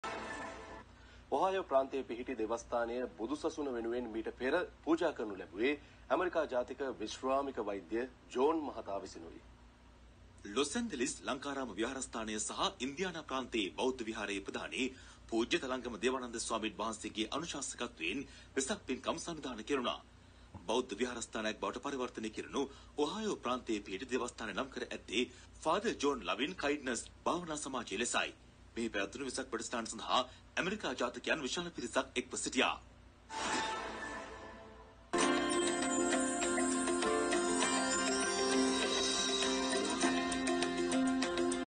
Loving Kindness Meditation Society Bryan Ohio USA
Vassa Aradhana Starting Vassa Rain Retreat Season. Thank you for all your kind supports. Sabbe Satta Bhavantu Sukhitata.

“Caratha bhikkave carikan, Bahujana hitaya, bahujana sukhaya."
"Go on walk, monks, for the happiness and well-being of all beings."
චරිථ භික්ඛවෙ චාරිකං
බහුජන හිතාය
බහුජන සුඛාය
අත්ථාය හිතාය
දේව මනුස්සානං